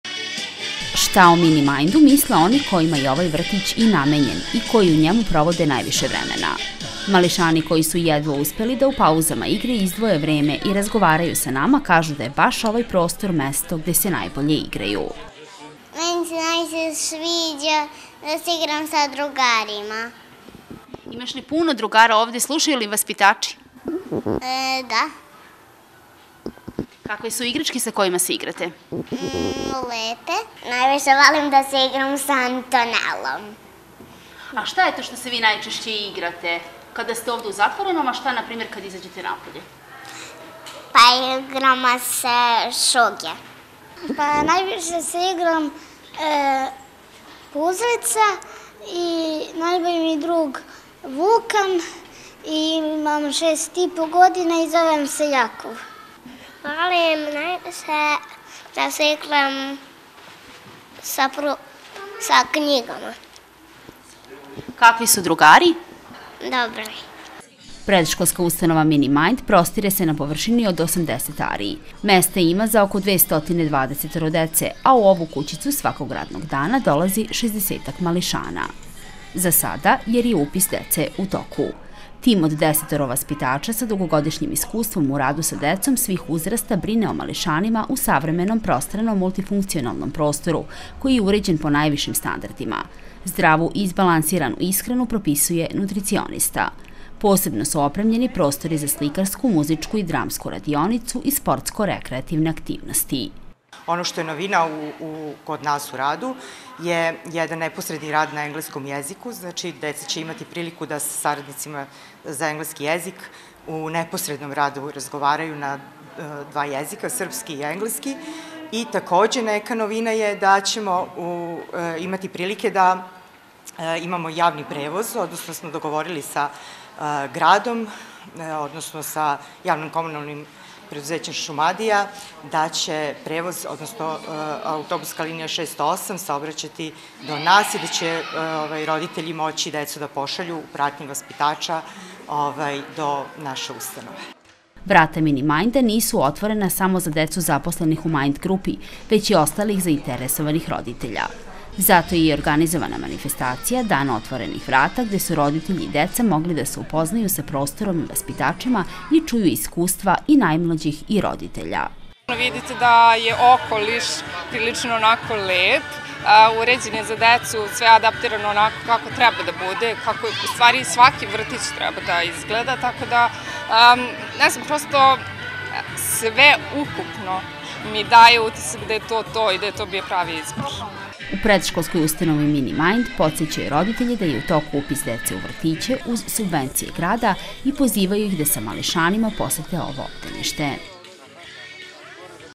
Mini Majnd – oaza za mališane
Predškolska ustanova Mini Majd proslavila je prvi rođendan.